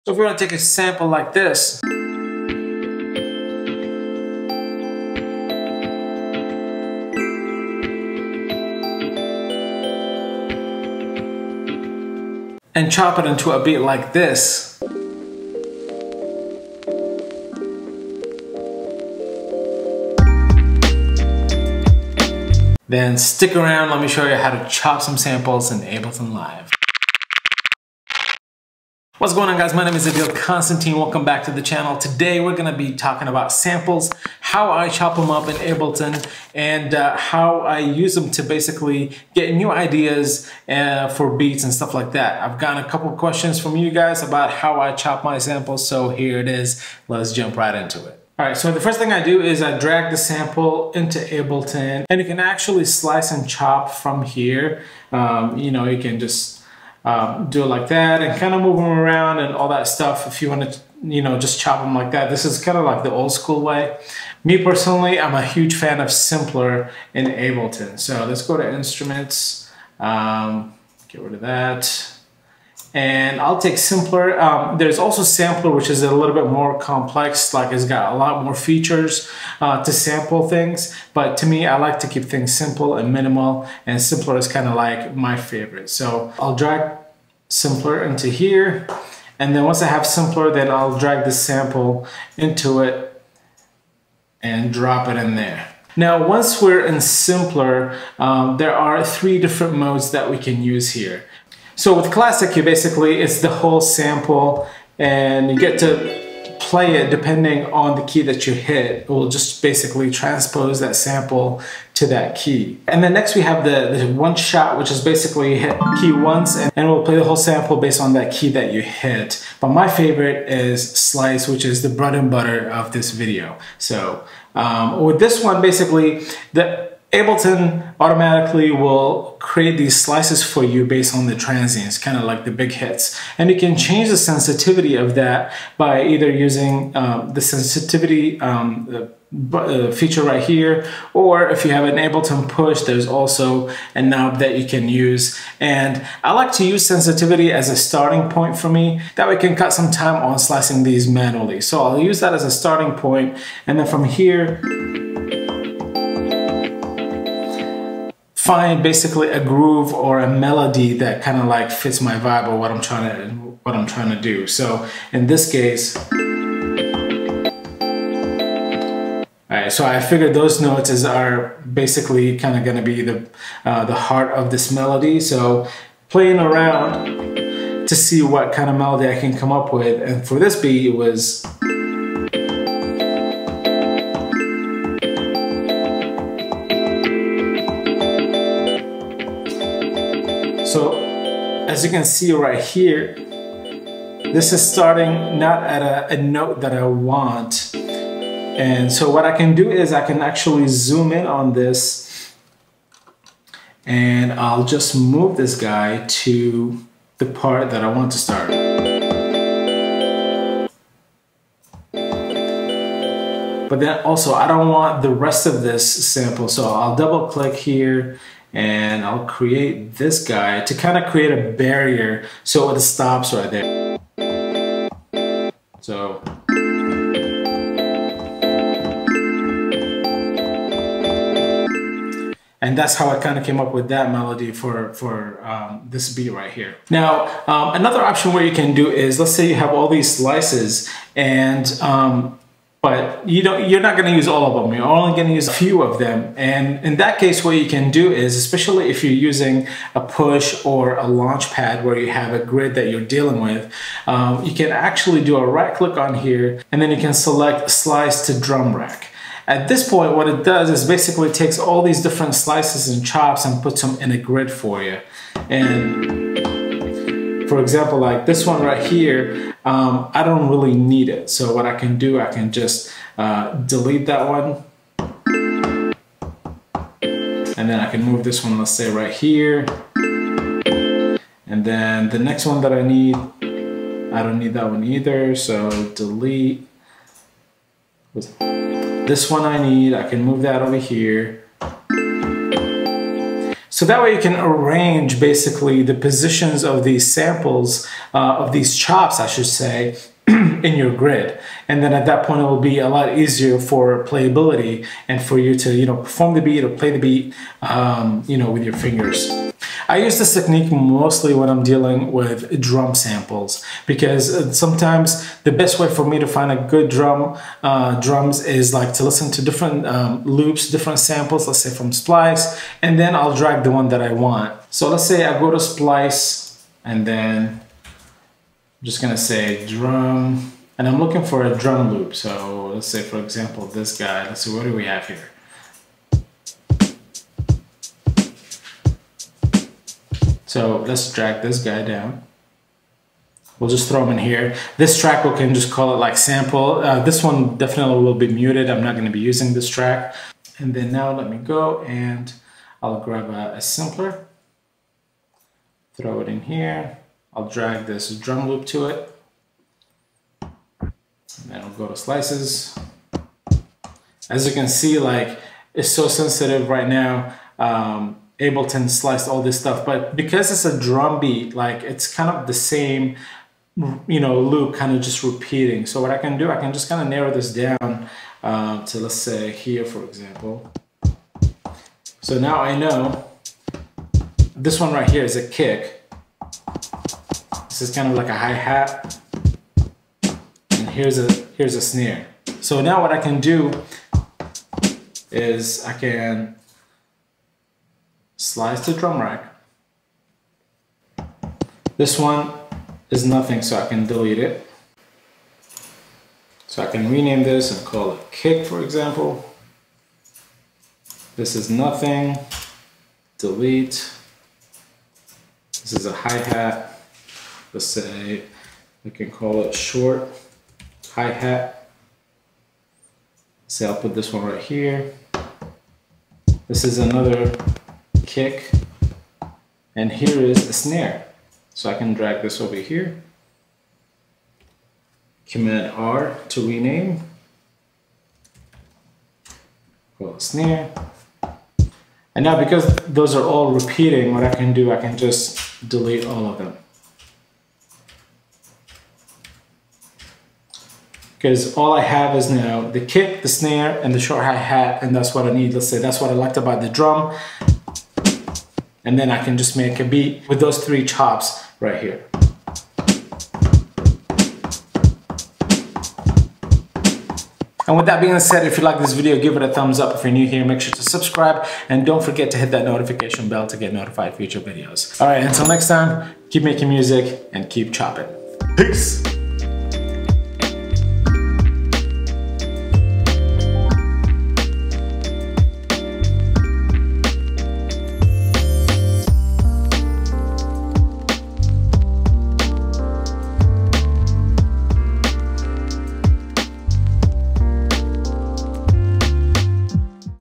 Chopping Samples in Ableton Live (Simpler Tutorial)
In this video, I go over I how I chop (slice) samples in Ableton Live using Simpler.

Amazon Music

Main Camera
Second Camera
Action Camera
Favorite Lens
Joby GorrilaPod